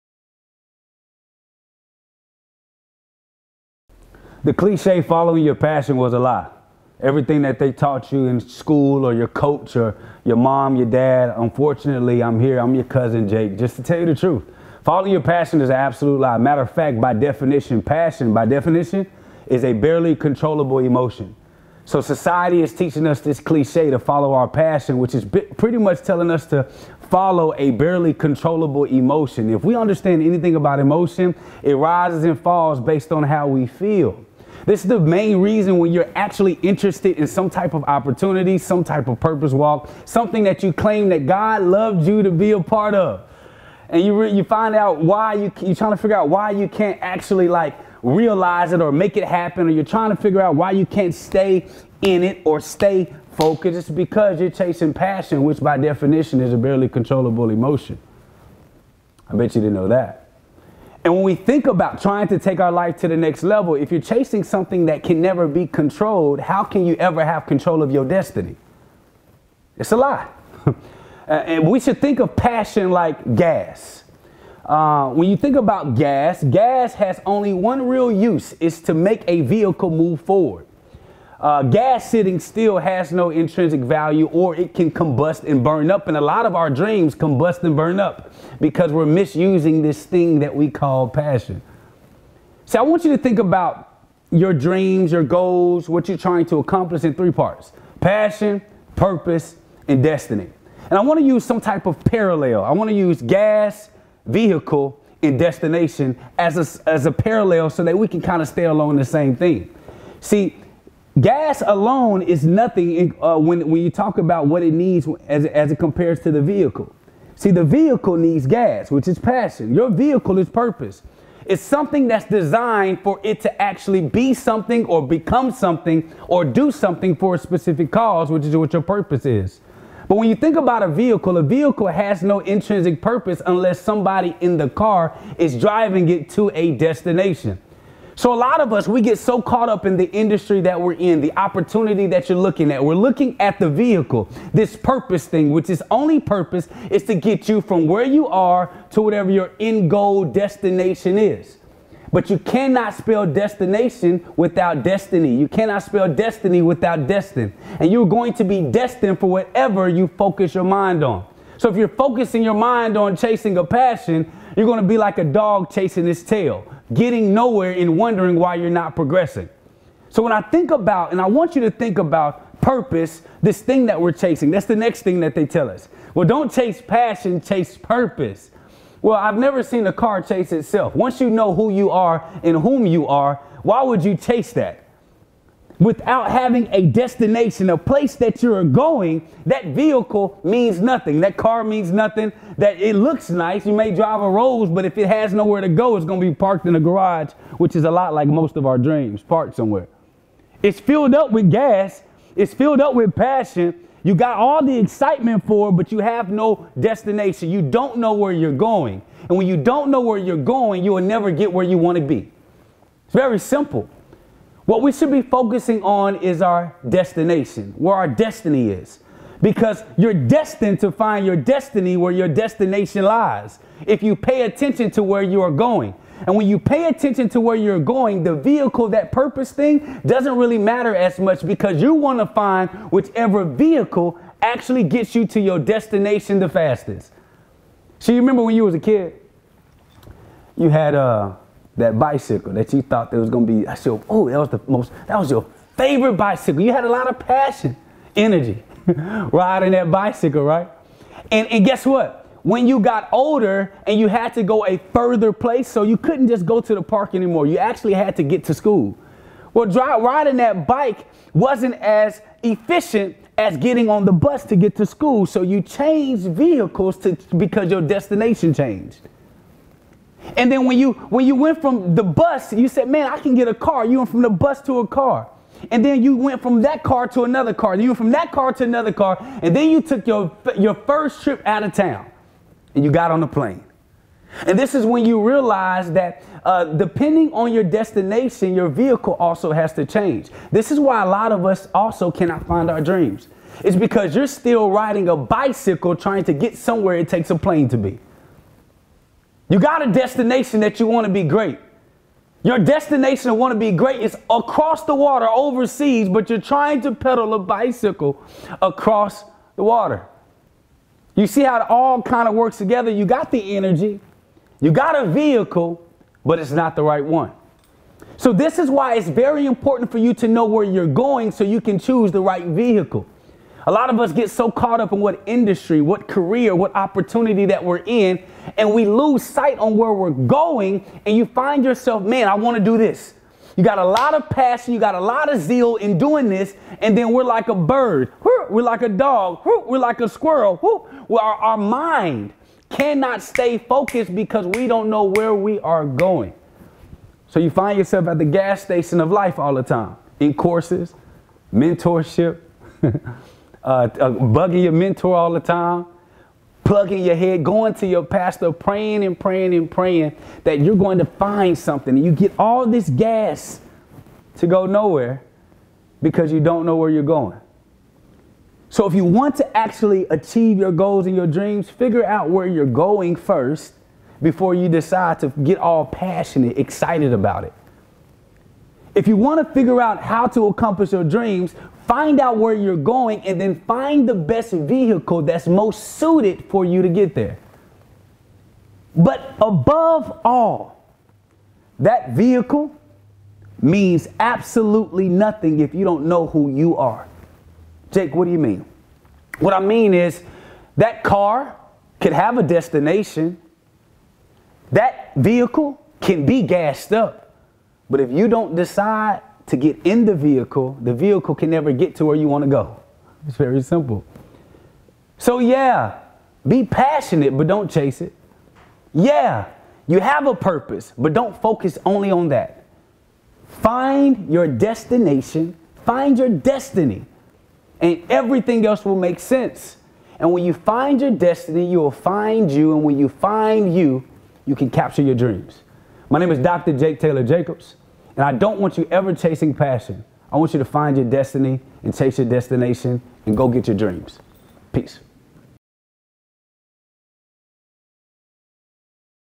Following Your Passion will burn your dreams. | Dr. Jake Tayler Jacobs | TEDxFishtown
Dr. Jake Tayler Jacobs takes us on an amazing journey of how following your passion can actually stop your dreams from becoming reality. Dr. Jake Taylor Jacobs is an American Entrepreneur and philanthropist with a specific fascination for banking. This talk was given at a TEDx event using the TED conference format but independently organized by a local community.